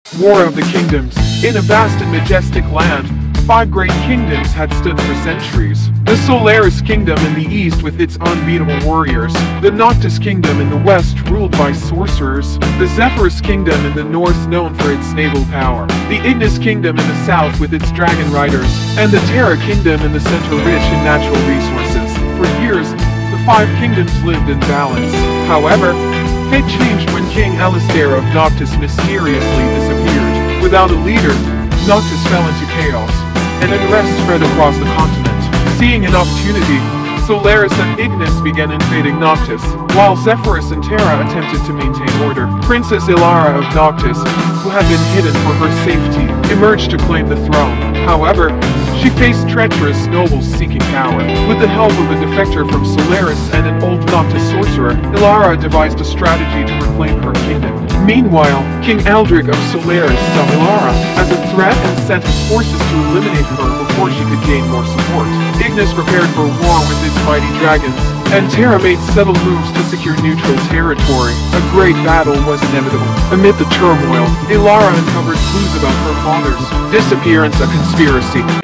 EPIC Kingdoms CLASH in Battle Royale Showdown!
Description:
"Artificial Intelligence (AI) has become one of the most influential technologies in the world. This video provides an in-depth explanation of what AI is, how it works, and its impact on various aspects of human life, including healthcare, education, business, and entertainment. Highlighting some of the most viral AI innovations worldwide, such as ChatGPT, Tesla Autopilot, and DeepMind, this video will open your eyes to the immense potential of AI for the future. Don’t miss this fascinating insight and be part of the technological revolution!"